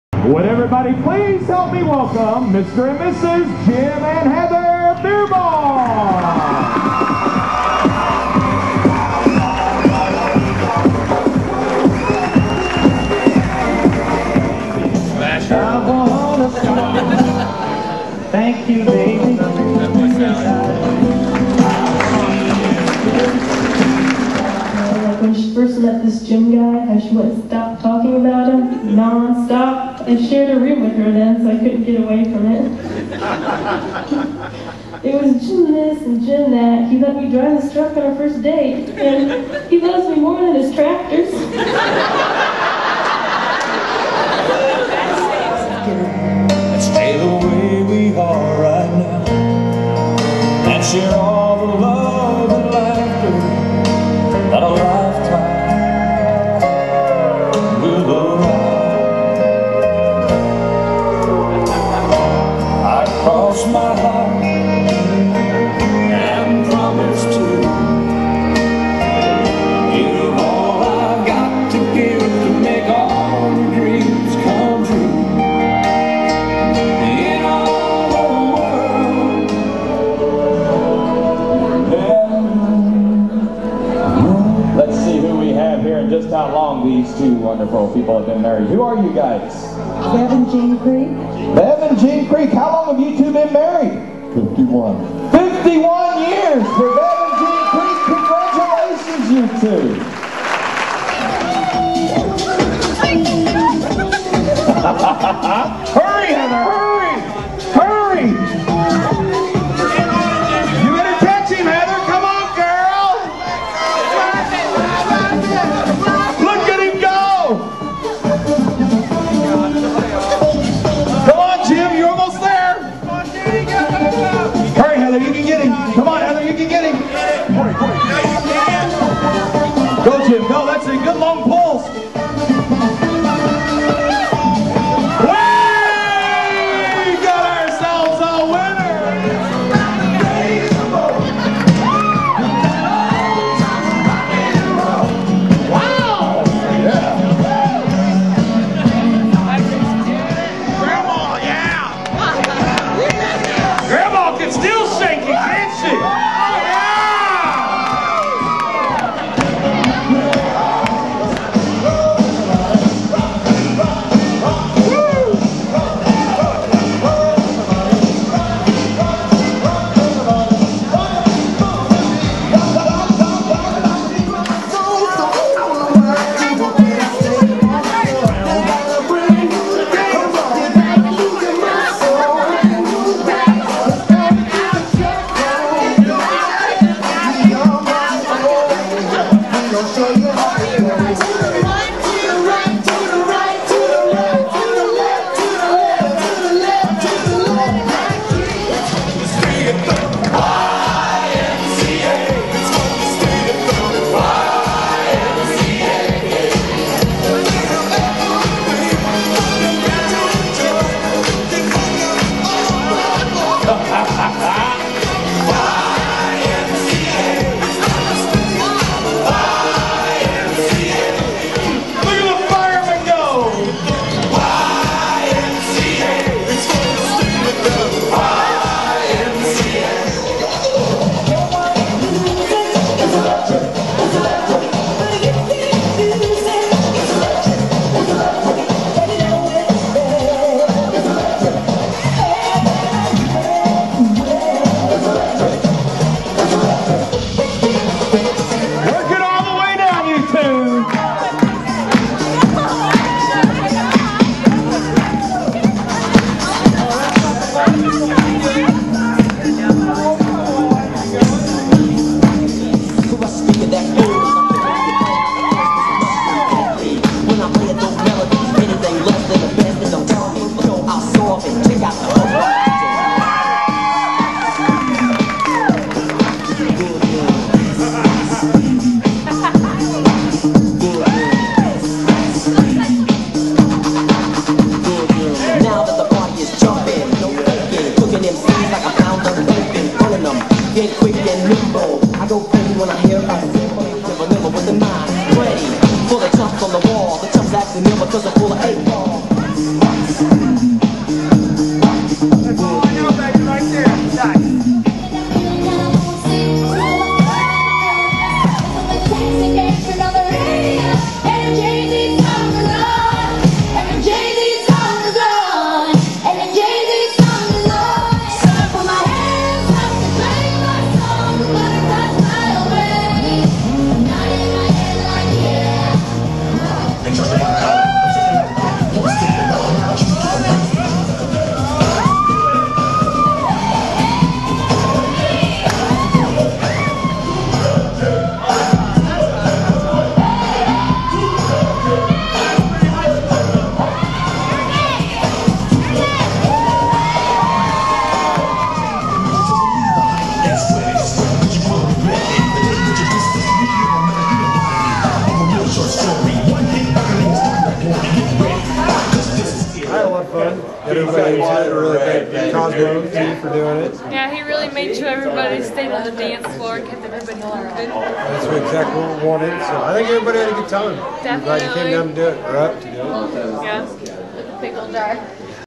Jim and Heather Berbaum August 21st, 2010.wmv
Jim and Heather Berbaum's Wedding reception. At Champaign IL. at Hawthorn Suites. On August 21st, 2010.